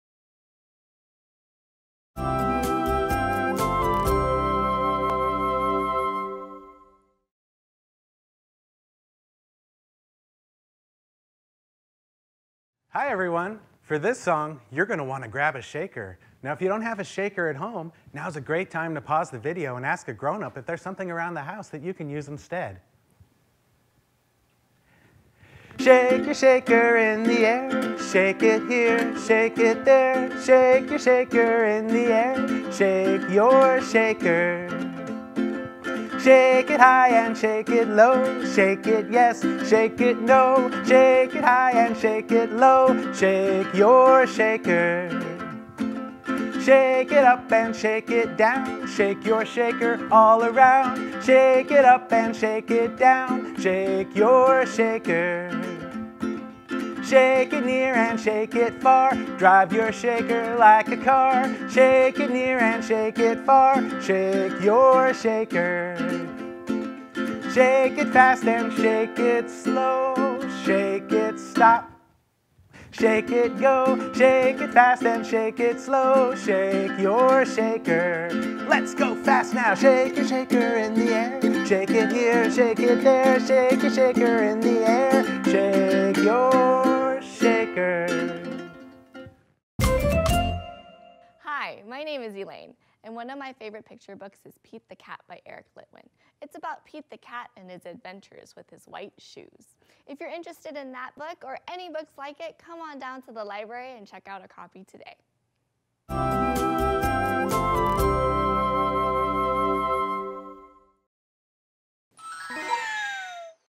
Video Storytime Snippet- 2/17/2021
Sometimes all you have time for is a Storytime Snippet and that's ok! We invite you to move, play, and sing along with your child while watching these short Online Storytime videos from the comfort of your own home.